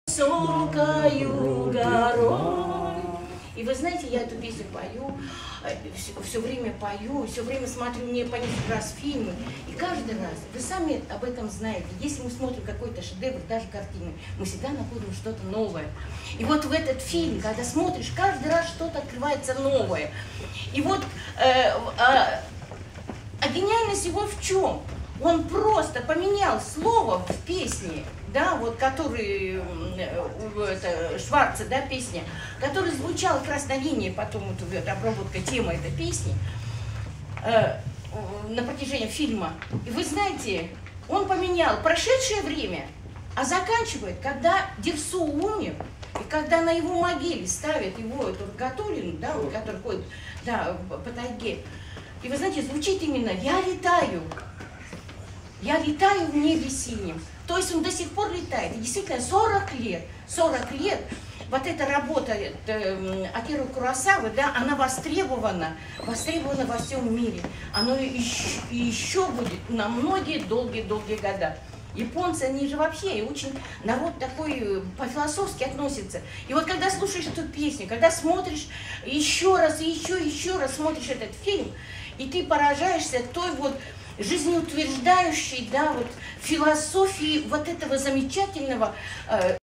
Светлана Мунзук о фильме "Дерсу Узала"
На вечере, посвящённом 40-летию фильма. Музей Востока (Москва) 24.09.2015